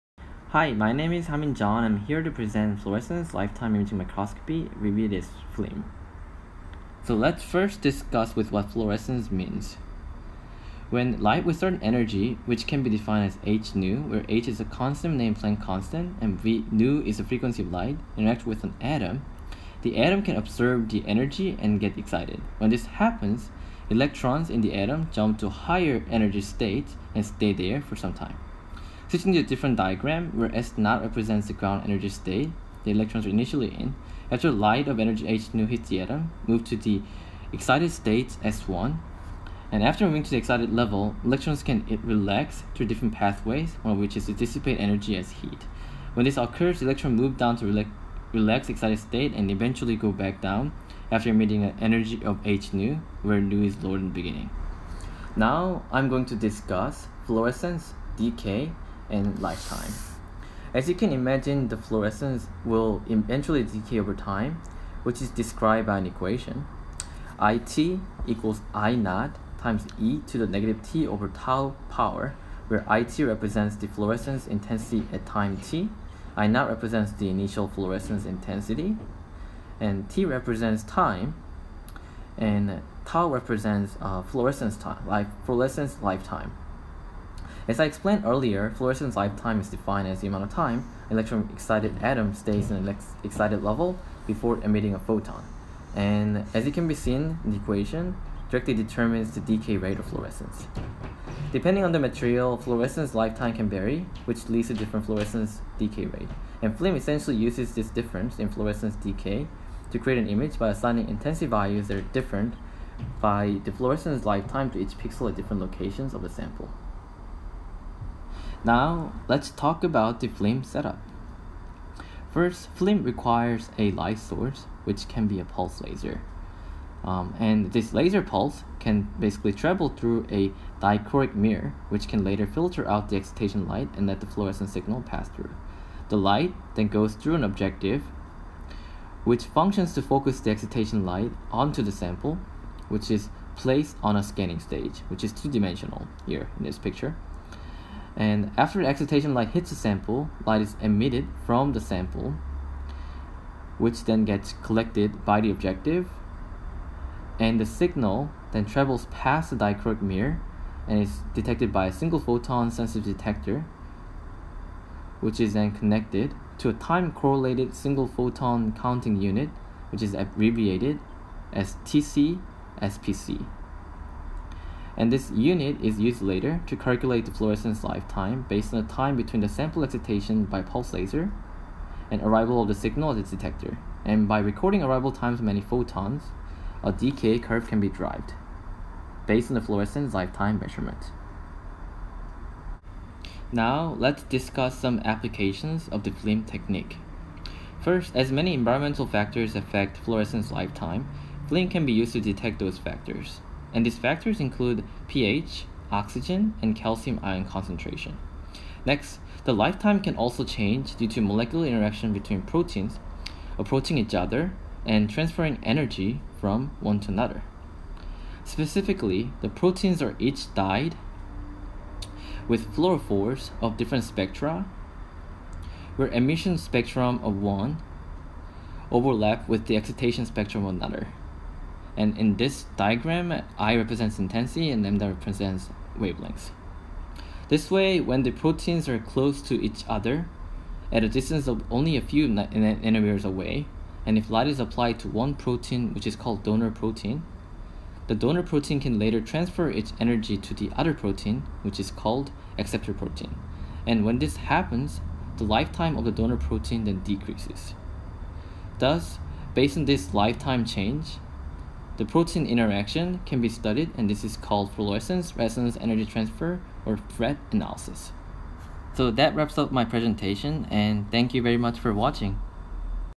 15_Fluorescence Lifetime Imaging Microscopy_HJeon
This video series was developed as part of the ELEC 571 Imaging at the Nanoscale course at Rice University. Inspired by the MinutePhysics videos, each video introduces a different aspect of nanoscale microscopy at undergraduate level for any STEM student. This particular video describes Fluorescence Lifetime Imaging Microscopy.